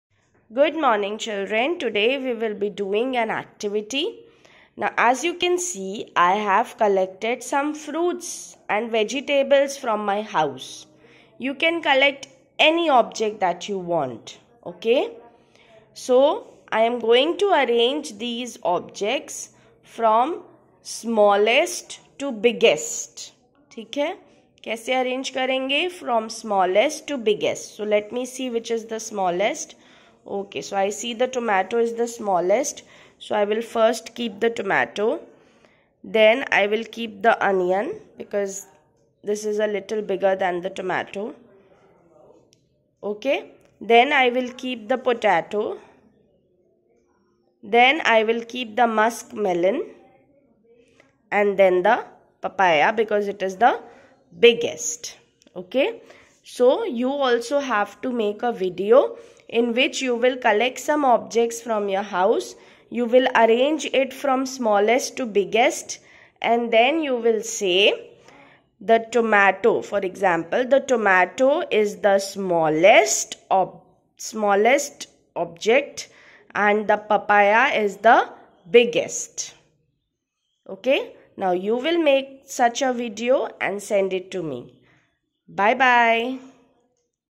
Math Activity 1
Arrange objects from smallest to biggest